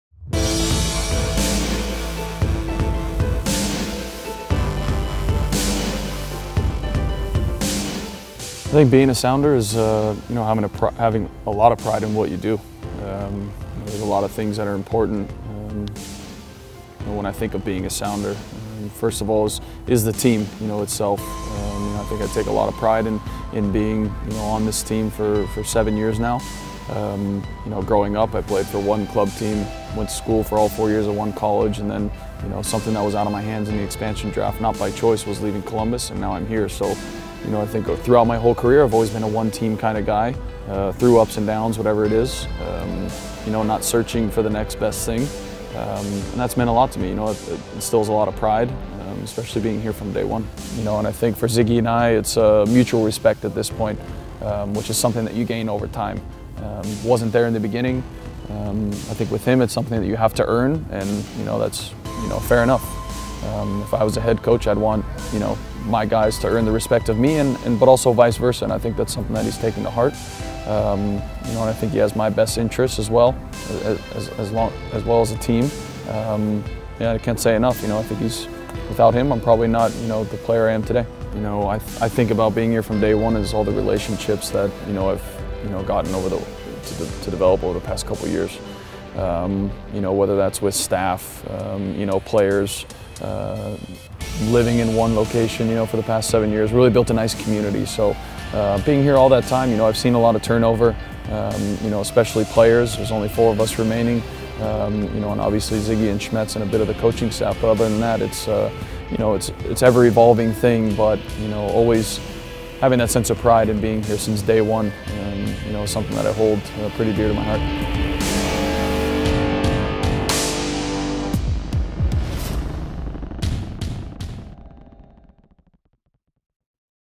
To Be A Sounder: Brad Evans, ever-present in the Emerald City
In this episode of "To Be A Sounder," Brad Evans discusses his allegiance to Seattle Sounders FC and what it has been like to play for the club since day one.